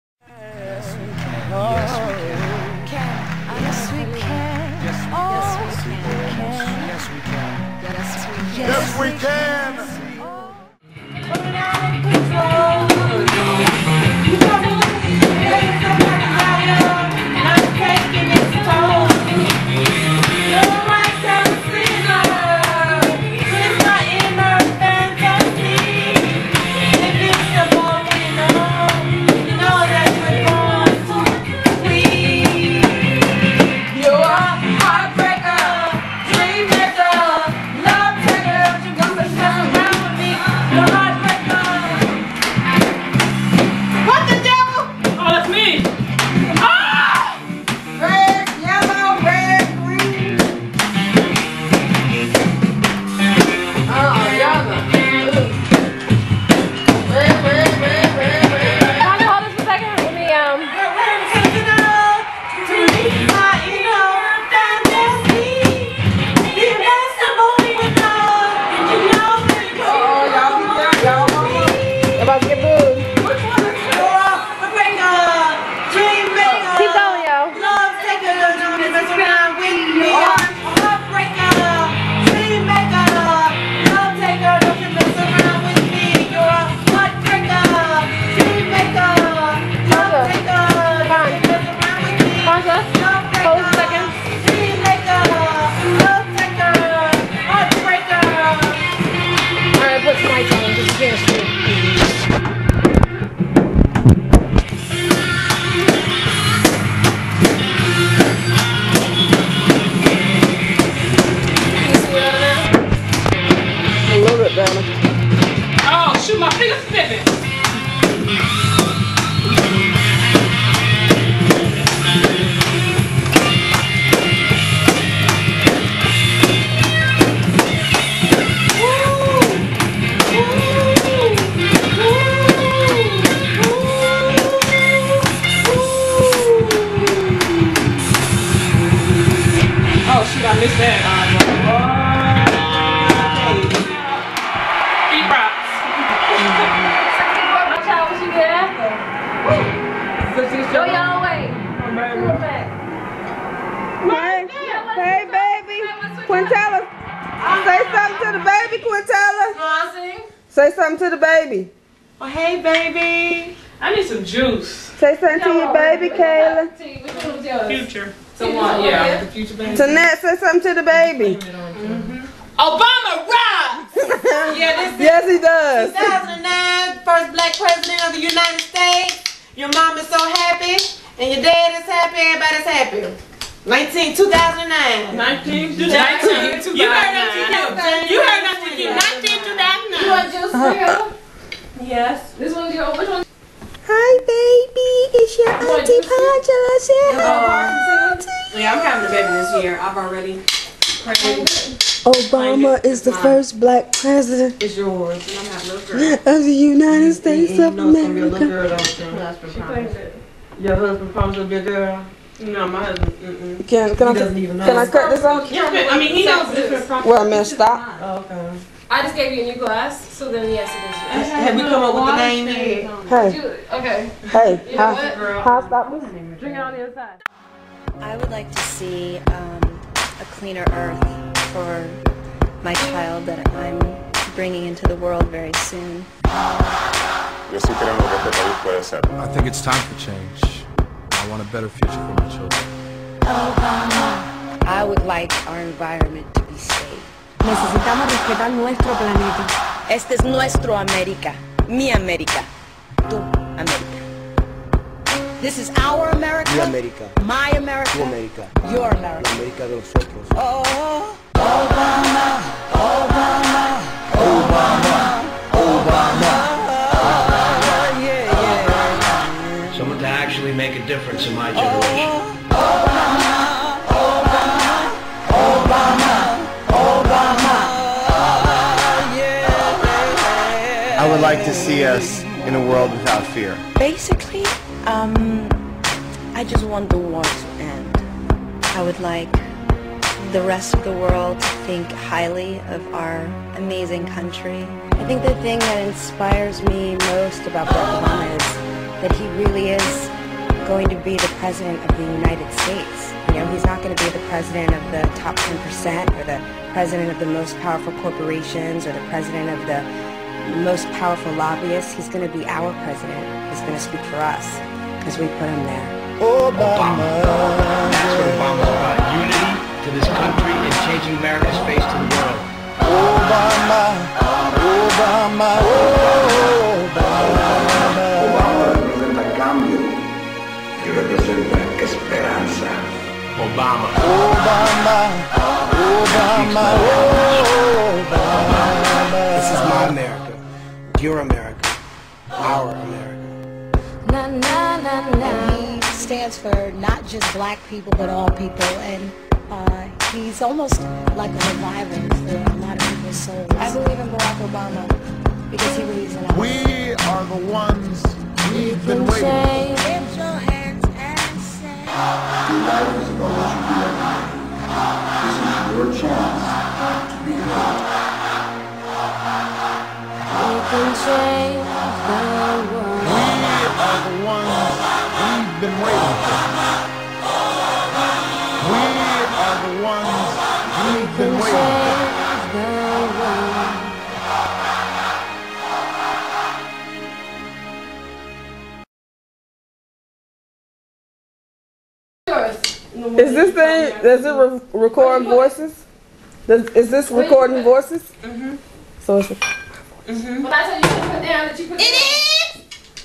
President Barack Obama PT2
Hello Friends & Subscribers, Have you wondered what other people did during the Presidential Inauguration? Well, here is what my wife's day went like. Thanks to all of the friends in the film: Zannette, Pongelia, Quintella, Tonette, Melissa, Ann, and Yolanda. The video was recorded by my wife Kayla. This video will show just a little of that important day across the nation.

Film Editor: Anthonyw20747